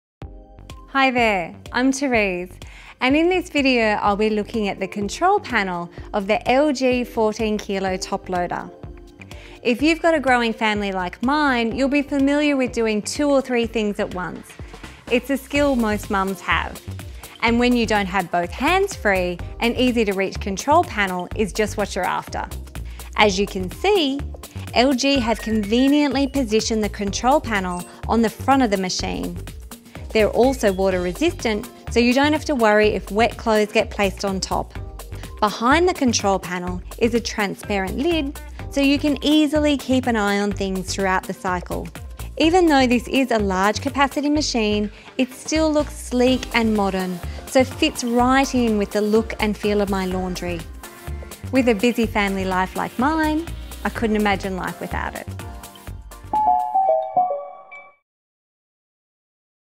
LG 14kg Top Load Washing Machine - Front Mounted Control Panel
The black, front mounted touch control panel and LED display makes selecting the correct washing program a breeze.